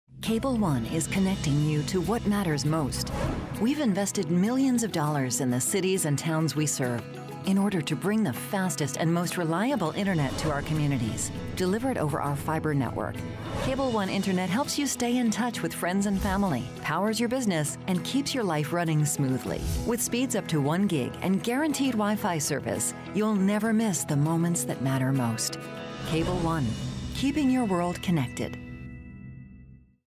Cable ONE - Investing in the Communities We Serve
Cable ONE is connecting our customers to what matters most. We’ve invested millions of dollars in the cities and towns that we serve in order to bring the fastest and most reliable internet to our communities.
Check out this video to see how we’ve invested in your community!